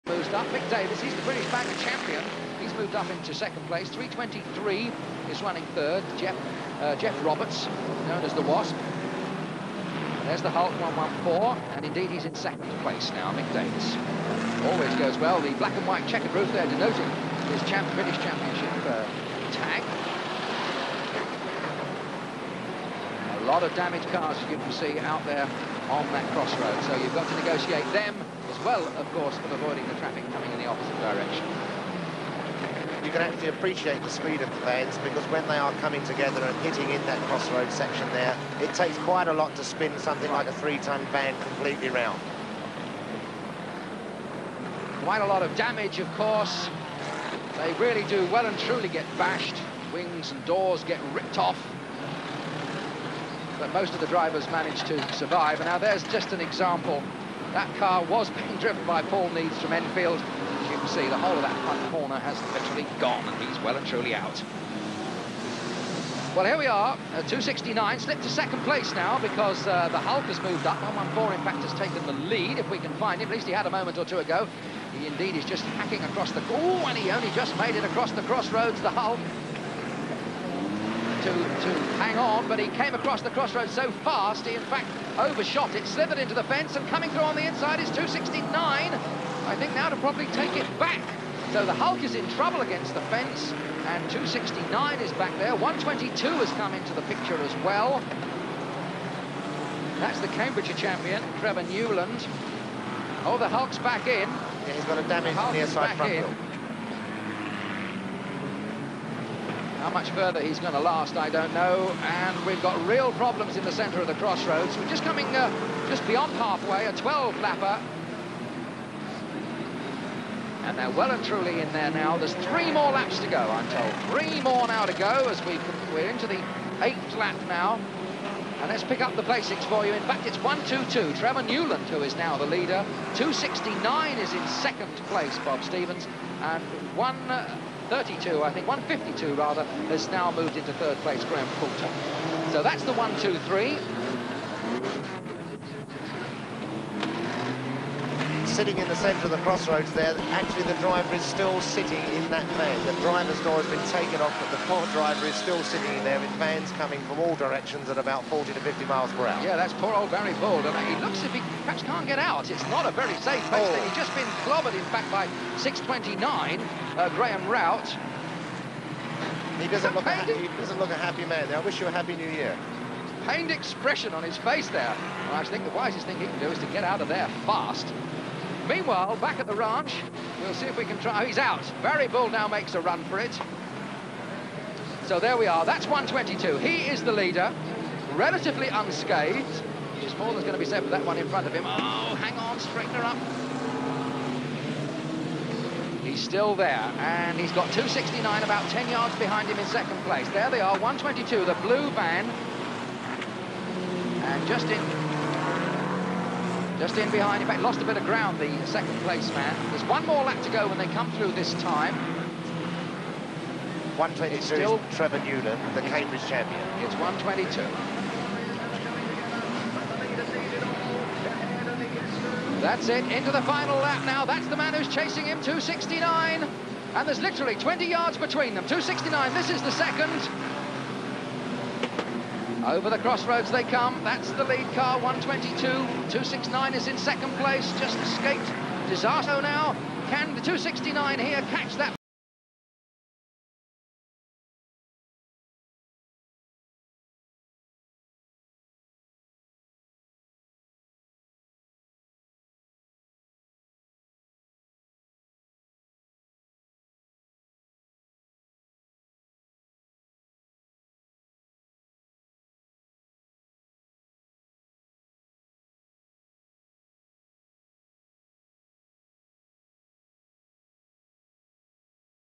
Banger Van racing at Wimbledon 1984 (part 2)
The 1984 Delivery drivers trophy on the Wimbledon figure of 8 track.